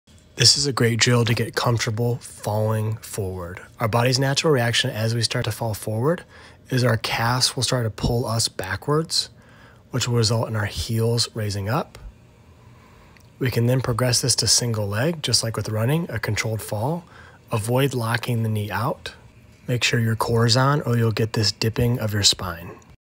Tight Calves Fix for Runners
Running is about FALLING forward, NOT propelling up and down with calves. This drill will teach you the proper mechanics for running.